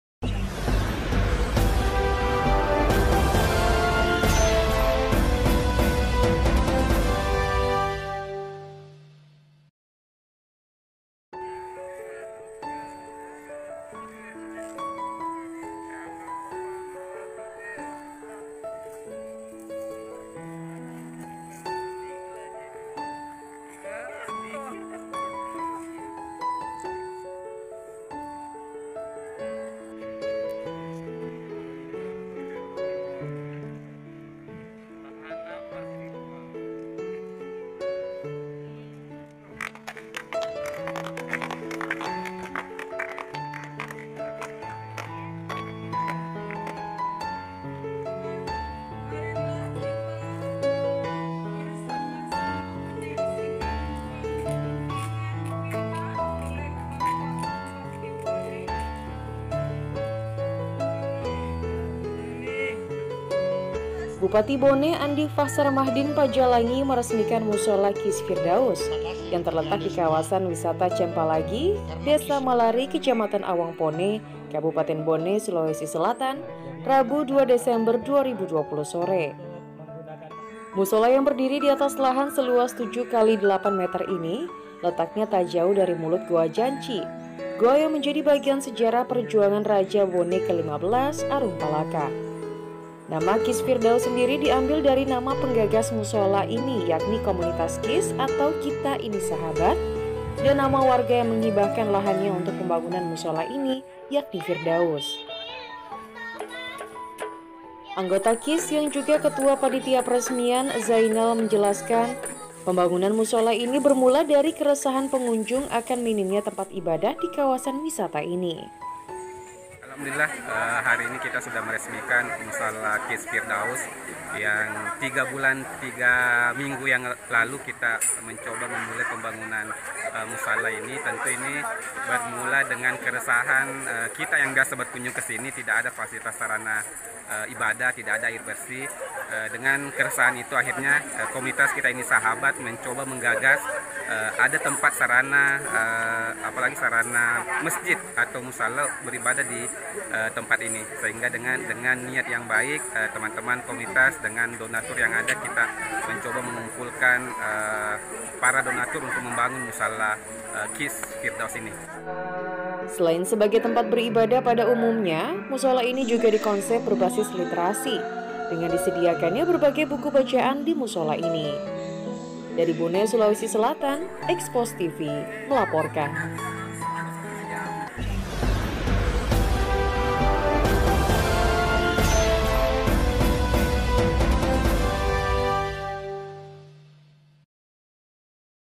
Peresmian Mushola KIS Firdaus Cempalagi
Bupati Bone Andi Fahsar Mahdin Padjalangi meresmikan mushola KIS Firdaus, yang terletak di kawasan wisata Cempalagi, Desa Mallari, Kecamatan Awangpone, Kabupaten Bone, Sulawesi Selatan, Rabu (2/12/2012) sore.

Mushola yang berdiri di atas lahan seluas 7×8 meter ini, letaknya tak jauh dari mulut goa janci, goa yang menjadi bagian sejarah perjuangan raja Bone ke-15, Arung Palakka.

Nama KIS Firdaus sendiri diambil dari nama penggagas musholla ini yakni komunitas KIS (Kita Ini Sahabat) dan nama warga yang menghibahkan lahannya untuk pembangunan Musholla ini, yakni Firdaus.

Anggota KIS yang juga ketua panitia peresmian, Zainal menjelaskan, pembangunan musholla ini bermula dari keresahan pengunjung akan minimnya tempat ibadah di kawasan wisata ini.

Selain sebagai tempat beribadah pada umumnya, Mushola ini juga dikonsep berbasis literasi, dengan disediakannya berbagai buku bacaan di Musholla ini.

Dari Bone Sulawesi Selatan
EKSPOS TV Melaporkan